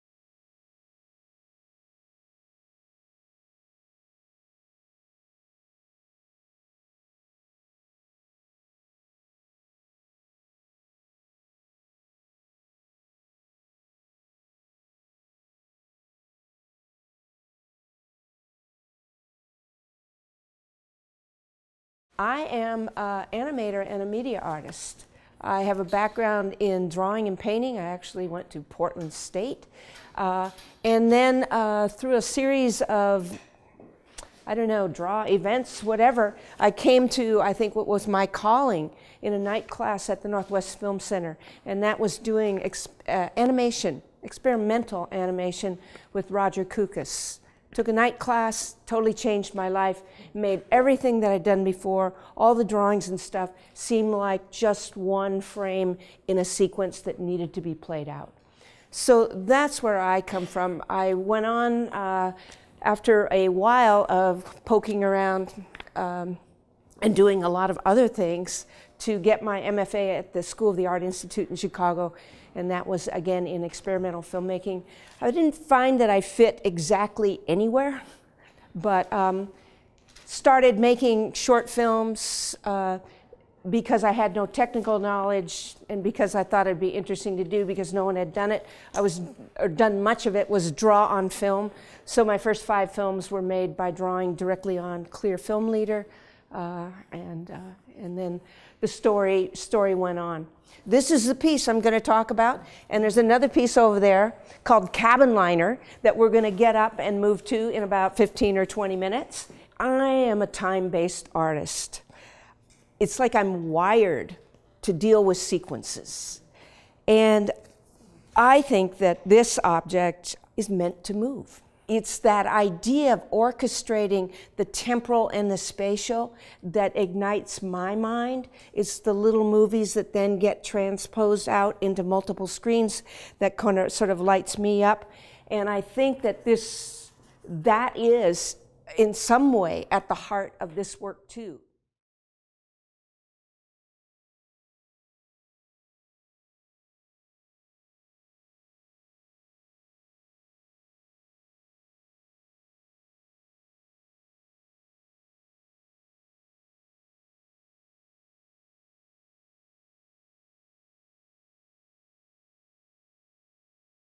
Artist Talk: Rose Bond (highlights)
In the February 2015 Artist Talk, Rose Bond discusses "Cabin Liner" (circa 1890), by an unknown Hidatsa artist and "Raven to Sun Transformation Costume" (1986) by Calvin Hunt, Jr. (Kwakwaka'wakw, born 1956).
Recorded February 12, 2015, at the Portland Art Museum.